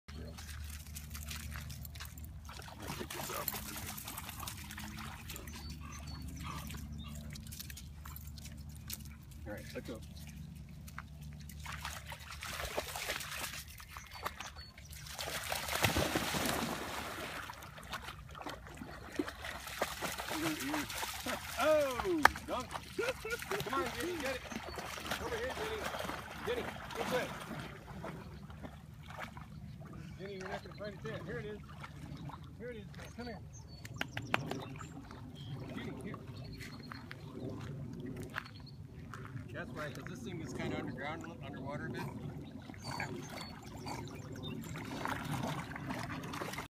Belgian Malinois puppy learning water retrieves.
4 month old Belgian Malinois Ginny learns to swim and retrieve in water. July 11, 2015.